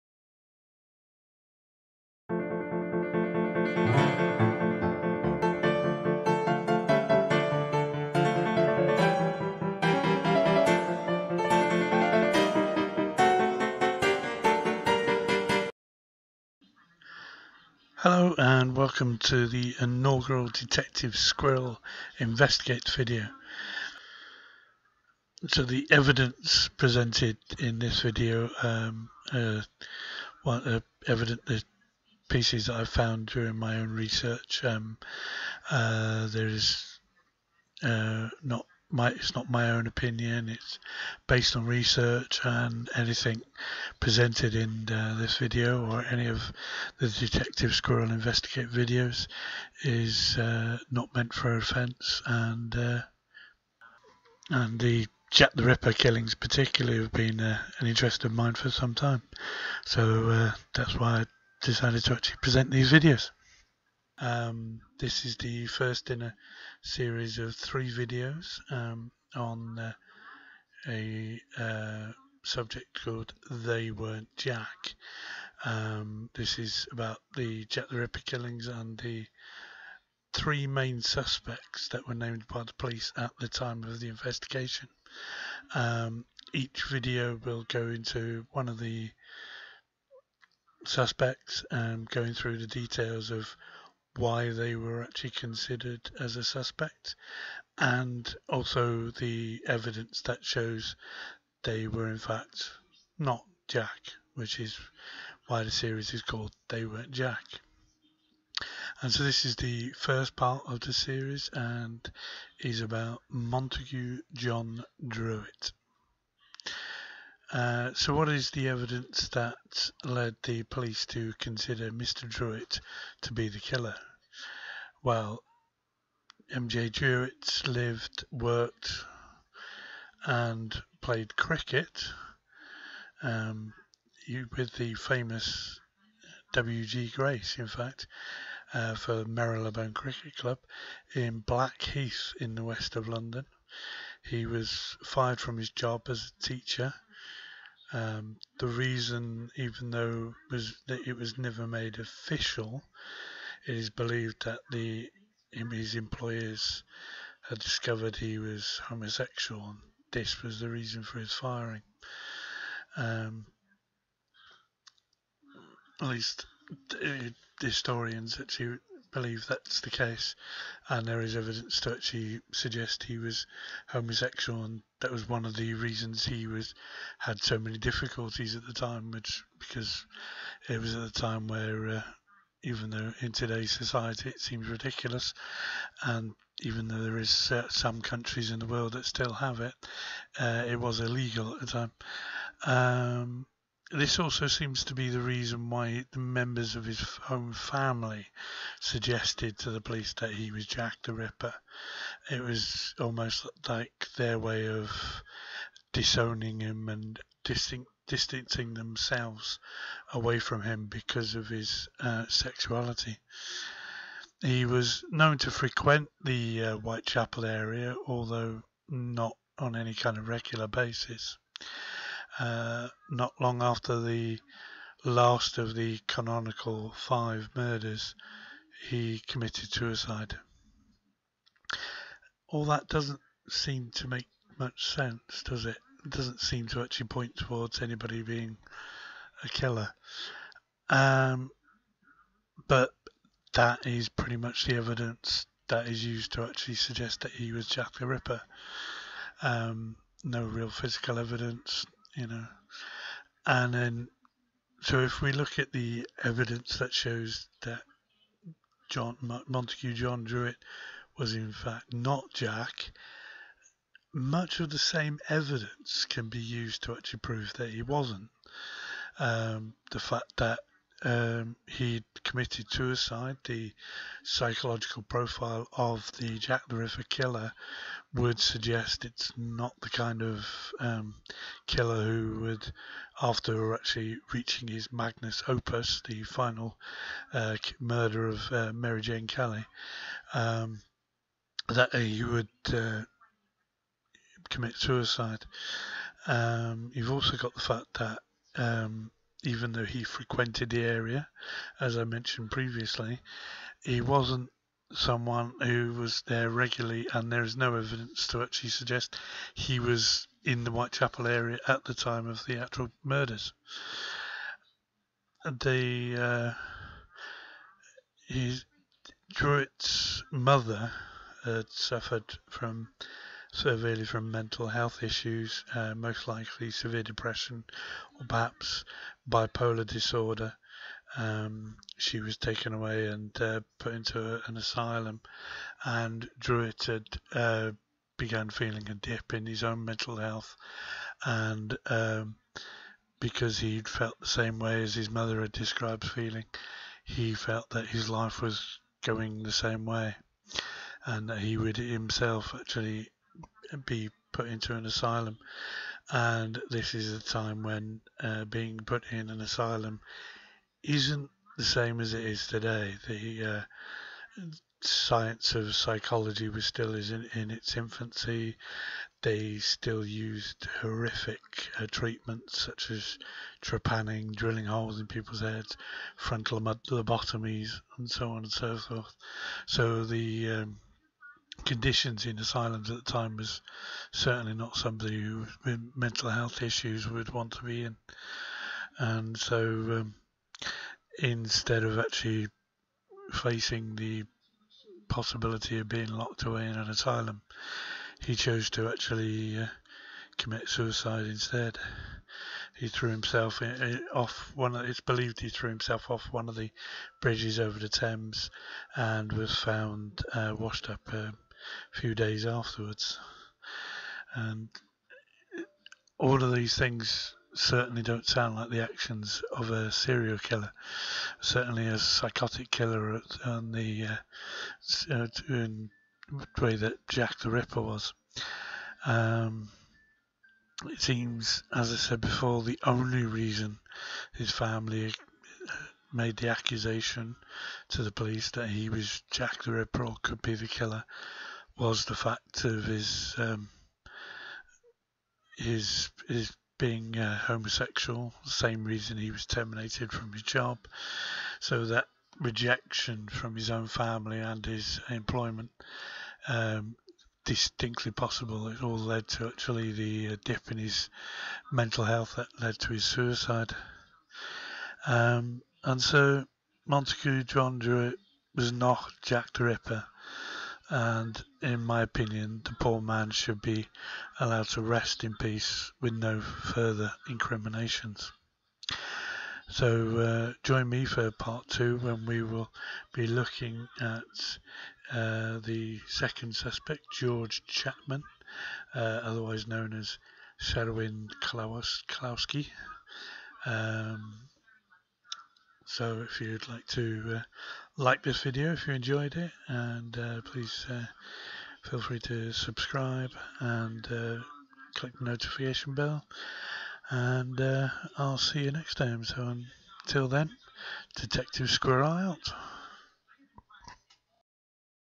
Detective Squirrel Investigates: They Weren't JACK! Part 1: Montague John Druitt.
Part 1 of 'They Weren't JACK' looking into evidence why Montague John Druitt was considered to be the Whitechapel killer and the evidence that proves that he was not Jack the Ripper.